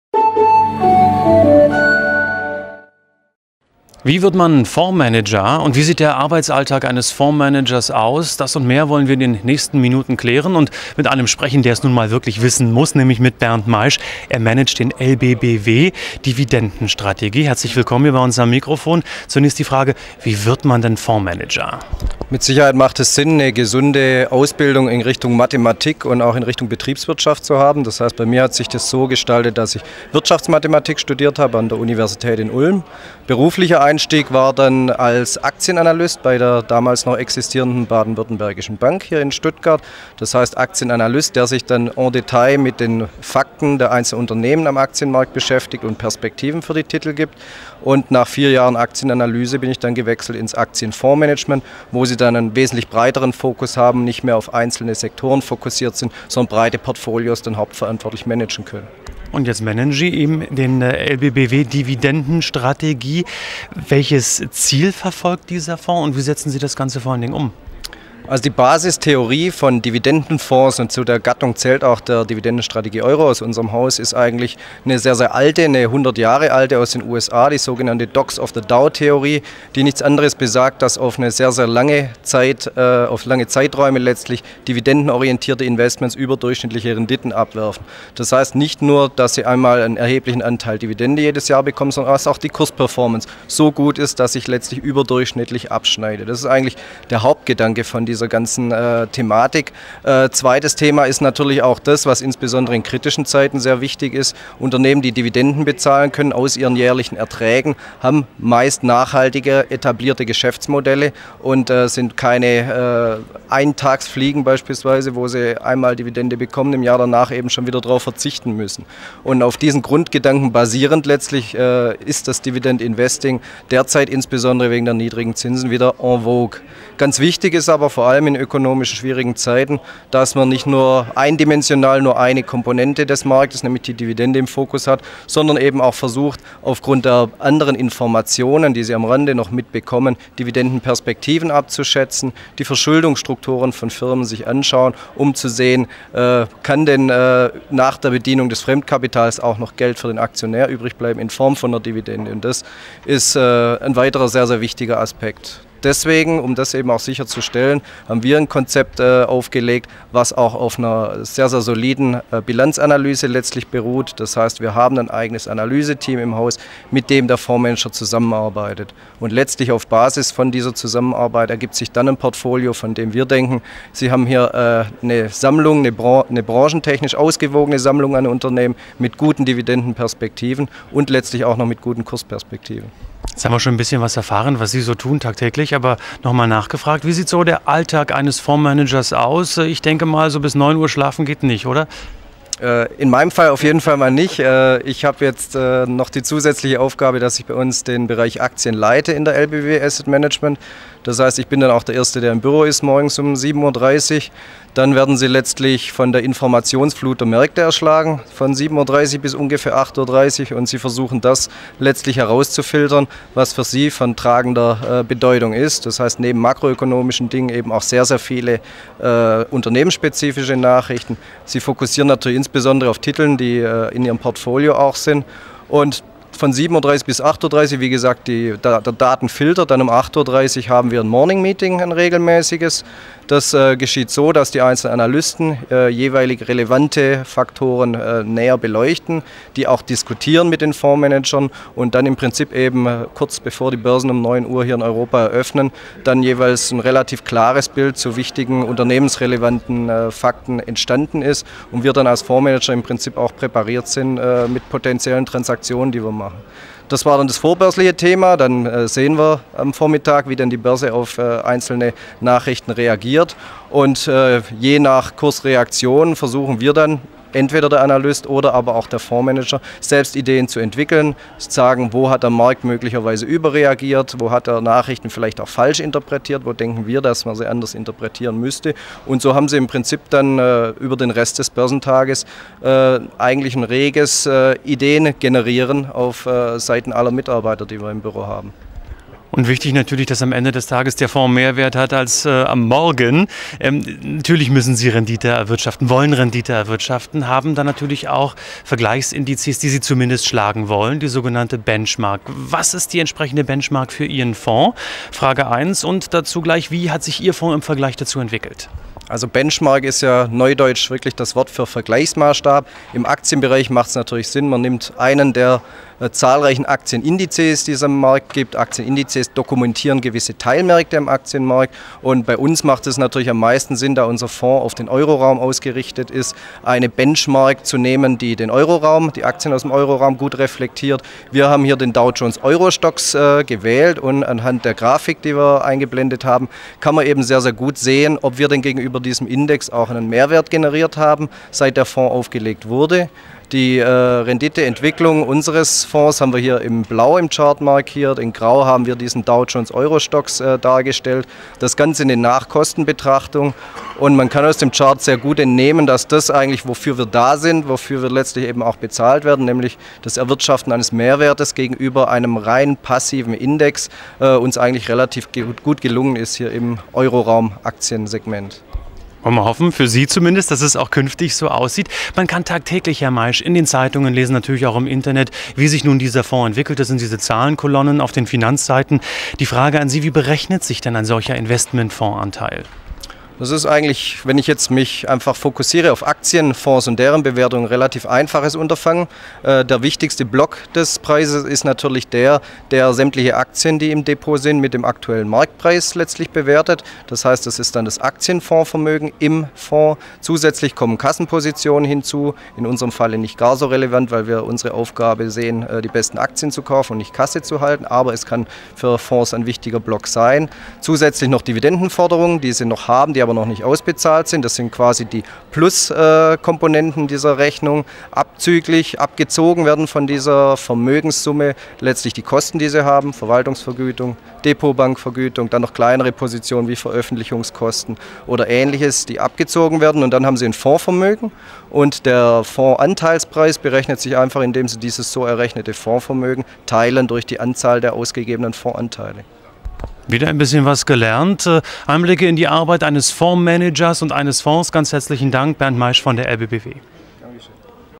Im Fokus: Der Job eines Fondsmanagers
Wie wird man Fondsmanager? Wie sieht ein normaler Arbeitstag eines Fondsmanagers aus? Und wie wird eigentlich ein Fondsanteil berechnet? Antworten darauf im Interview von Andreas Franik mit Berndt Maisch - er managt den LBBW Dividendenstrategie und gibt einen Einblick in seinen Job.